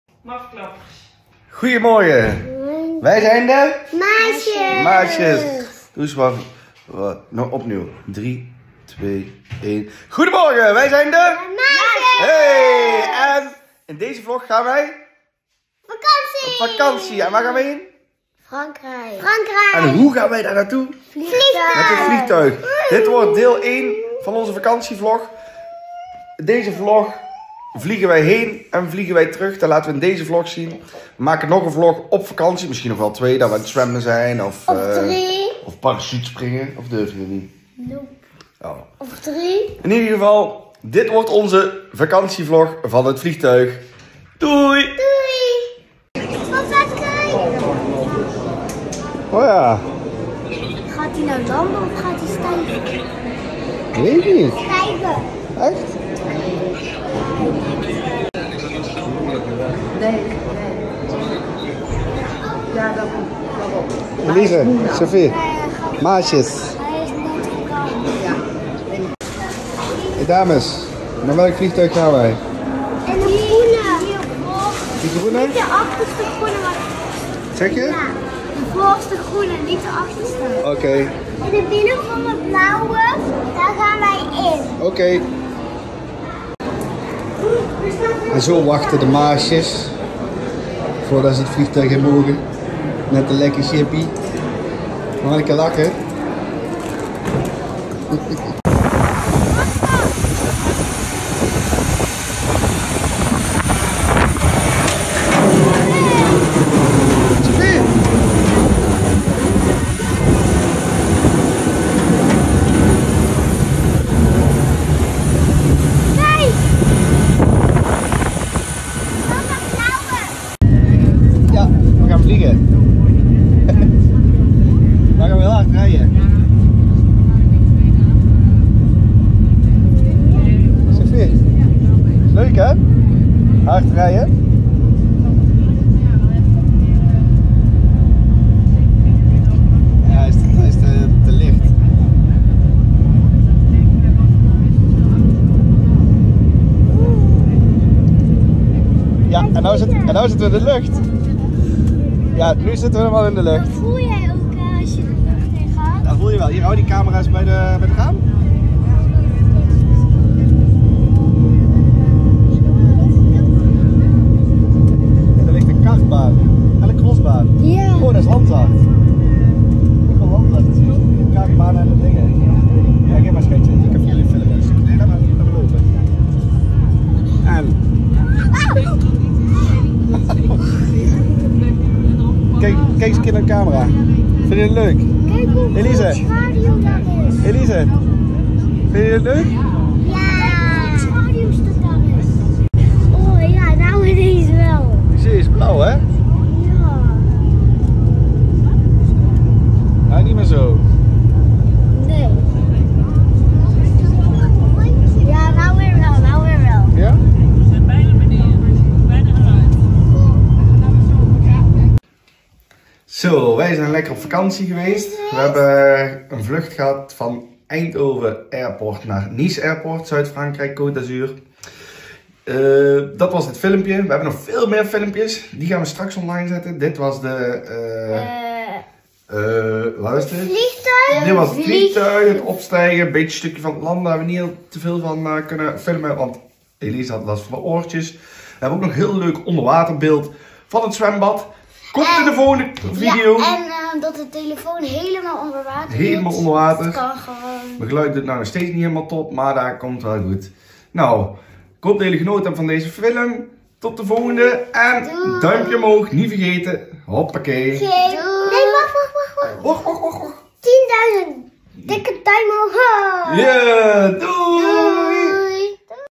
De Maasjes, in het vliegtuig naar Frankrijk
Beste kijkers, vandaag gaan we vliegen.

We vliegen van Eindhoven airport naar Nice airport.
We hebben van onze vakantie in Frankrijk nog veel meer leuke video's die we later aan uploaden.

Van de daling hebben we wat minder beeld omdat Elize een beetje last van haar oren heeft gekregen tijden het laden.

Heel veel kijk plezier!

Heb je iets leuks wat we kunnen doen? laat het ons weten in een comment!